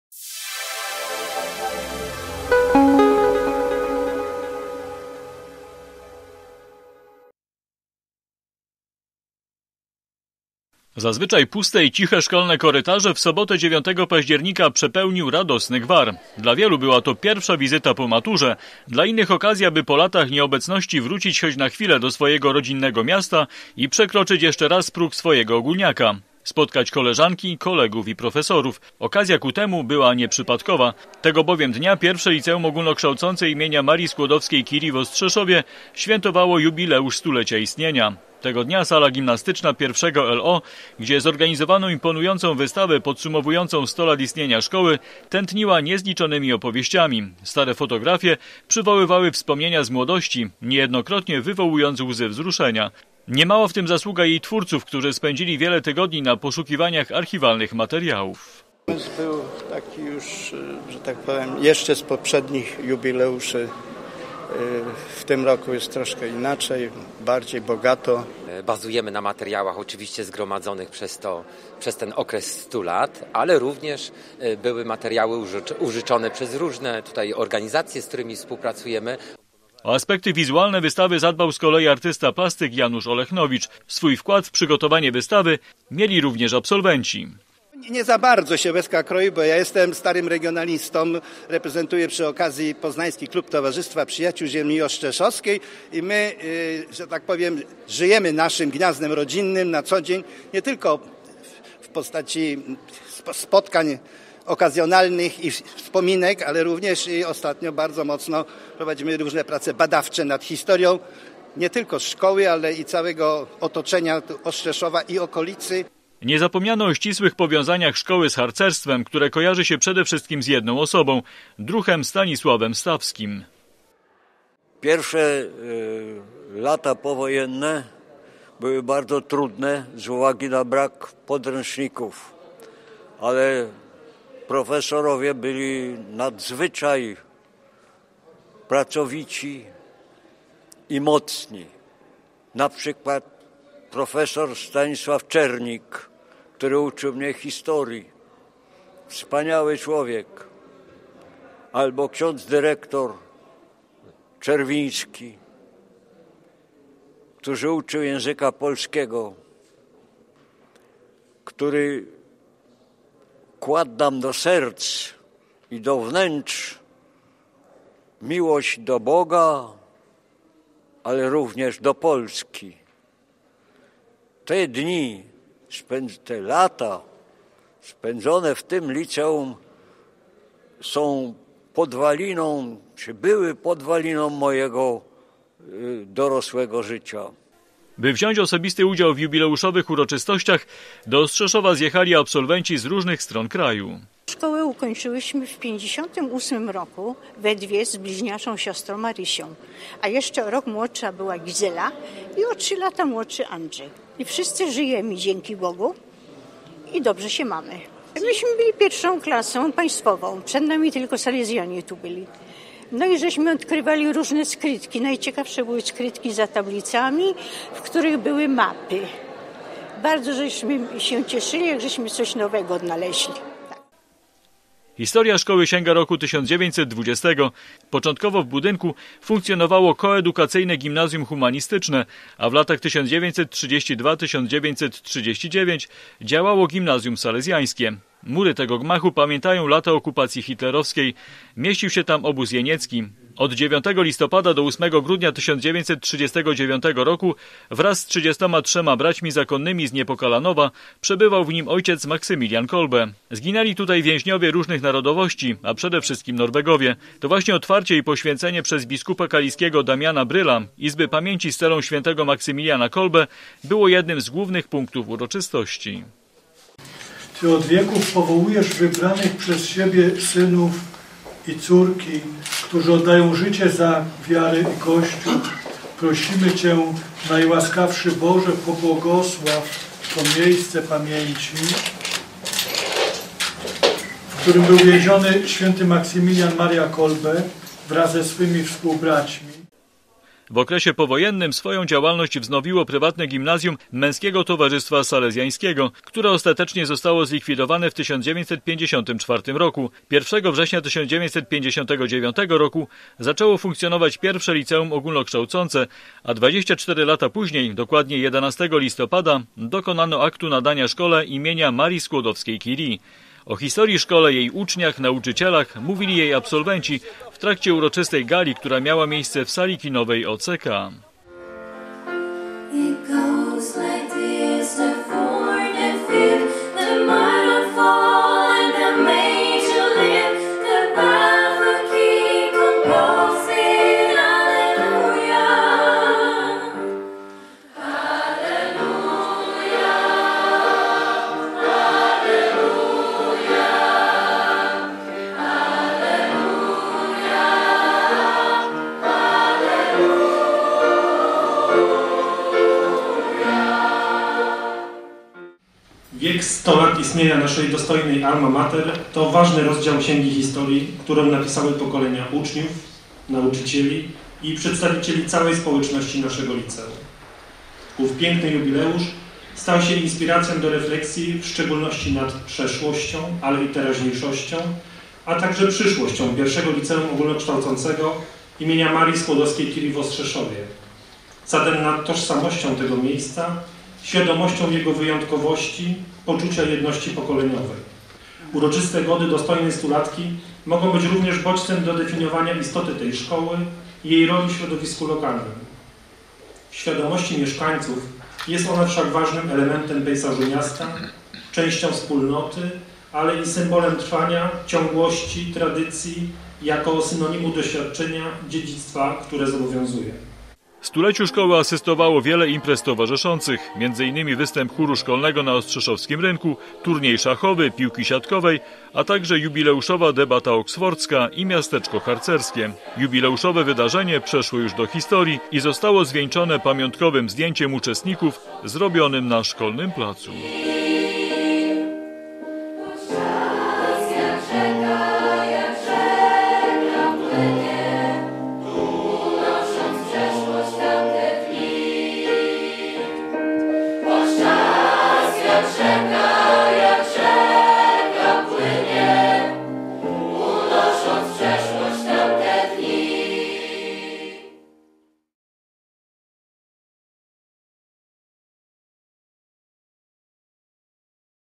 I Liceum Ogólnokształcące w Ostrzeszowie świętowało 100-lecie istnienia
Zazwyczaj puste i ciche szkolne korytarze w sobotę 9 października przepełnił radosny gwar. Dla wielu była to pierwsza wizyta po maturze, dla innych okazja, by po latach nieobecności wrócić choć na chwilę do swojego rodzinnego miasta i przekroczyć jeszcze raz próg swojego ogólniaka. Spotkać koleżanki, kolegów i profesorów. Okazja ku temu była nieprzypadkowa, tego bowiem dnia I Liceum Ogólnokształcące im. Marii Skłodowskiej-Curie w Ostrzeszowie świętowało jubileusz 100-lecia istnienia.